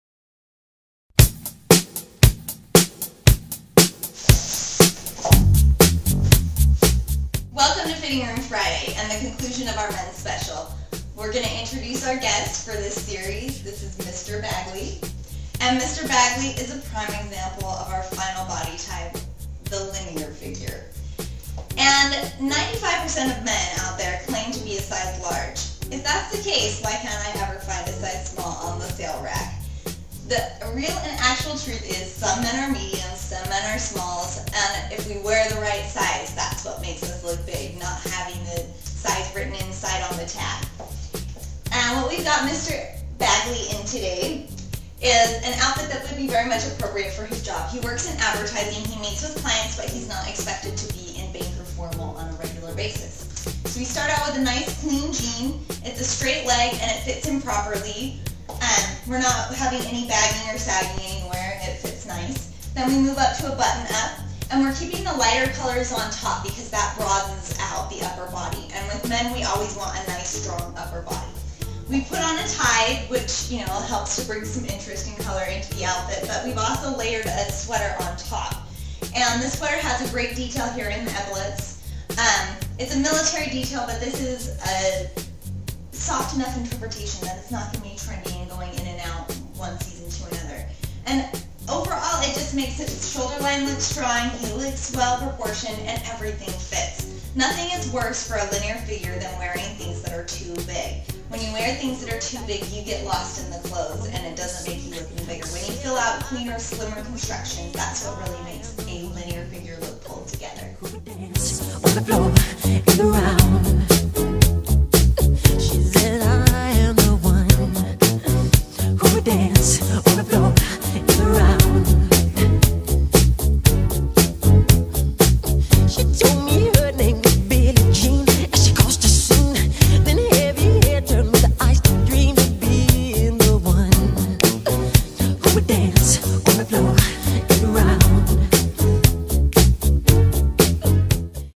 Fitting Room Friday: Male Body Type
Reachel Bagley of Cardigan Empire demonstrates how men can dress for their body type.